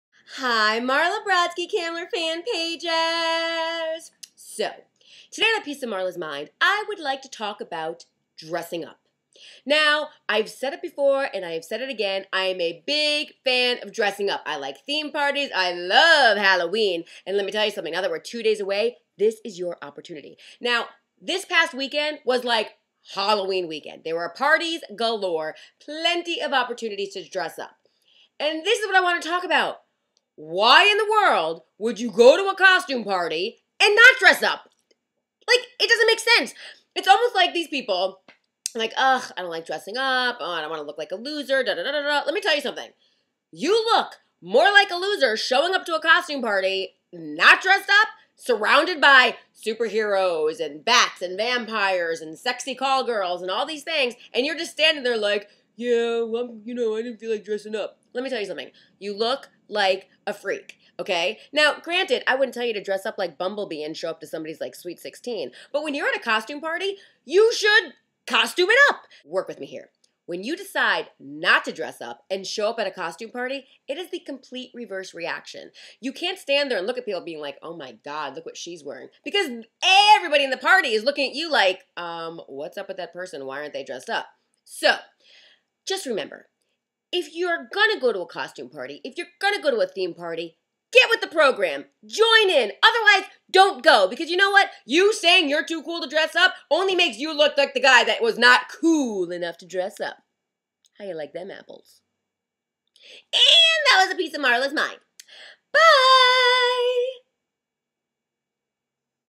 A PIECE OF MARLA'S MIND INSTALLMENT: 117
Somewhat EXCLUSIVELY for the Marla Brodsky Kamler Fan Pagers on Facebook!  Today Marla shares her views on Dressing Up